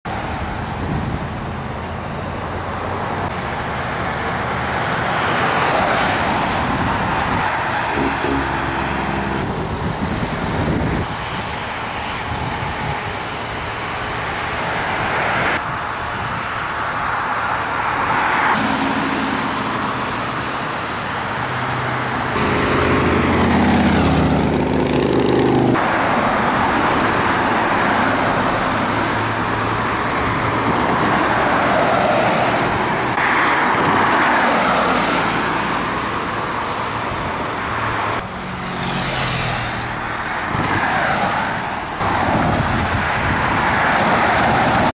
Single Vehicle Rollover by Moncton Airport
Just before 10am on highway 15 west bound lane heading into Moncton , across from the Moncton Airport ,  a single vehicle rollover sending a woman in her 40's to hospital with unknown injuries.  The vehicle took out one of the CN crossing lights and 1 lane of the highway has been blocked for over a hour. Dieppe Fire Dept and Codiac RCMP & Dist 11 RCMP responded to the call . Road conditions were dry at time of the accident.